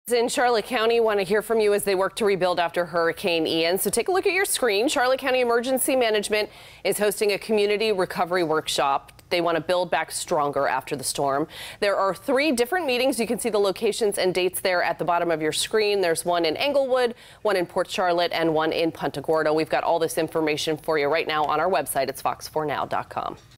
Community Recovery Workshops hosted in Charlotte County
Charlotte County residents will have the opportunity to have their voices and ideas heard in creating a Long-Term Recovery Plan.